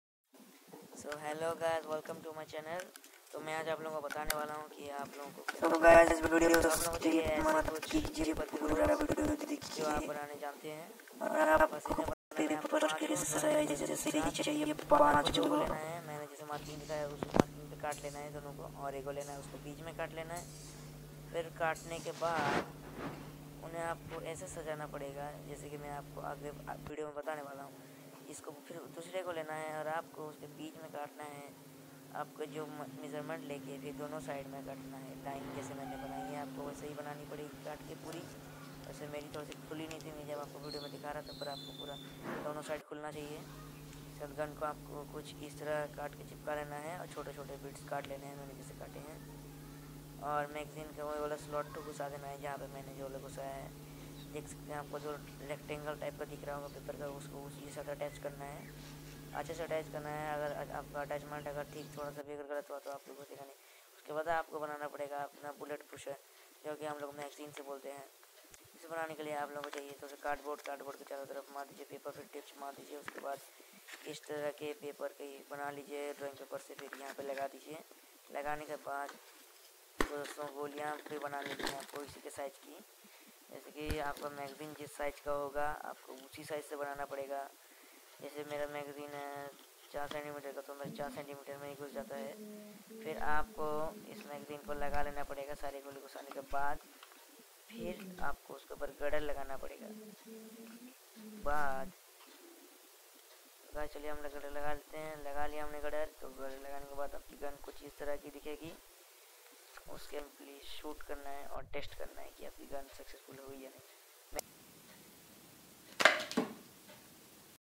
How to make a gun out of paper |that shoots bullets at home| magzine
To make this you will need

7.drawing paper
1. cellotape
2.rubber band
1. pen knife
1.glue

It is very easy.